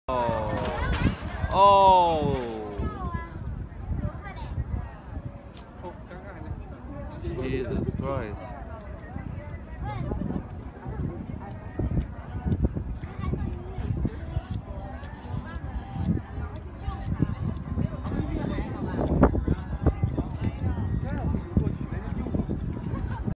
boat crashed to the USS Midway aircraft carrier museum in San Diego
a sail boat crashed to the USS Midway aircraft carrier museum in San Diego today